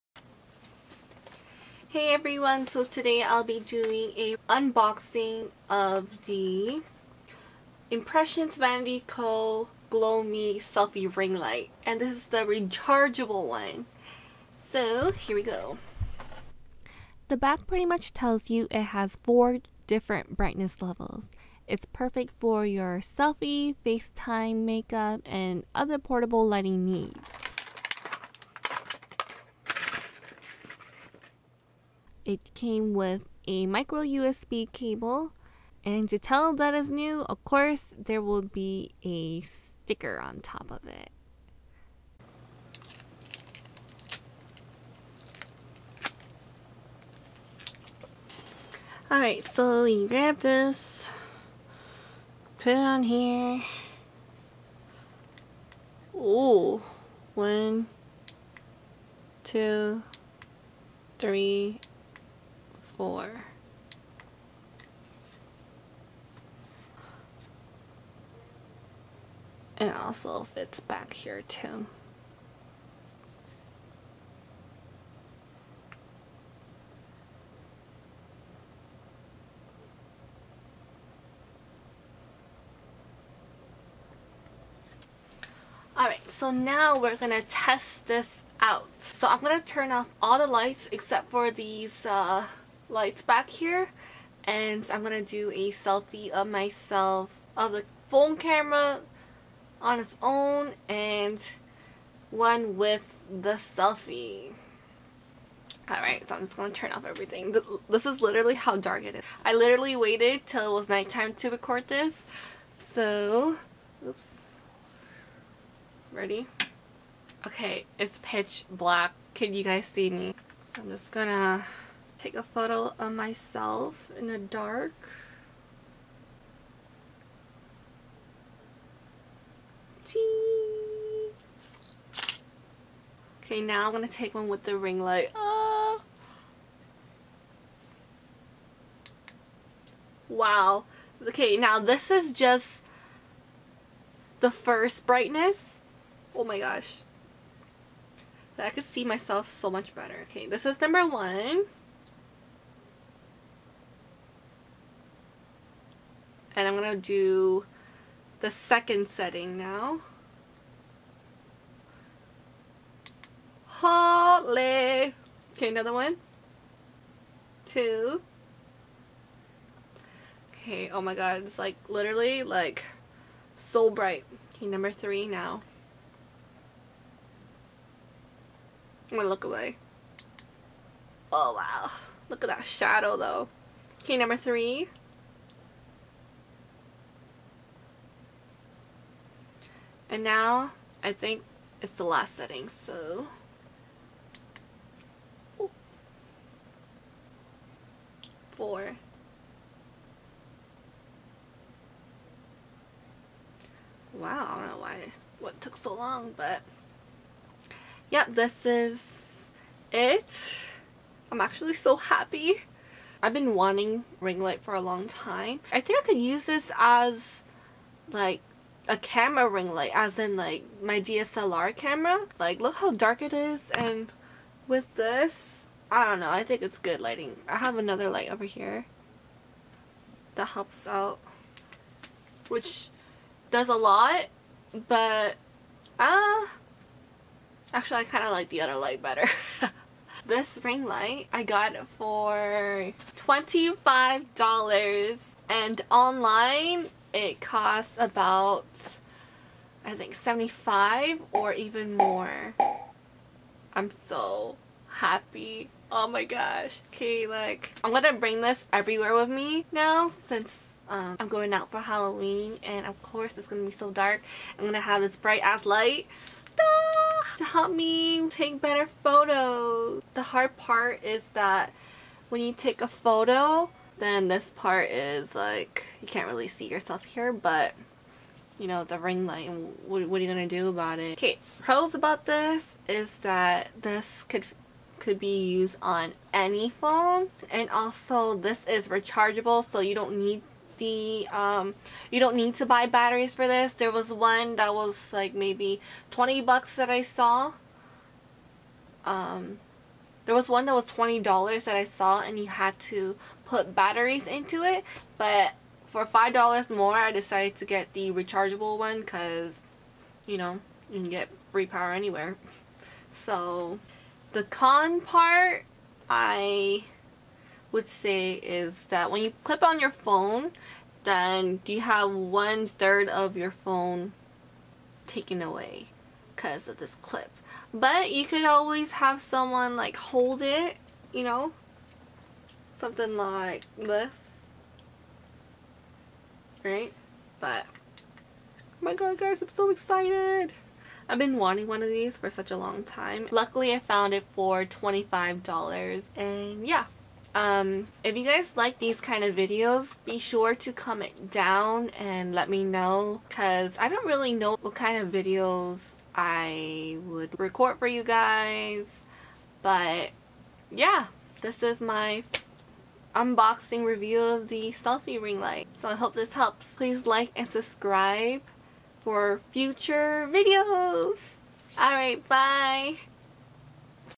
Unboxing IMPRESSION vanity GLOWMe selfie ring light (rechargeable) 😲
Bright white LEDs with 4 brightness levels
perfect for all your selfie,facetime & other needs
Comes with USB cable.

Easy just clip on and use! :)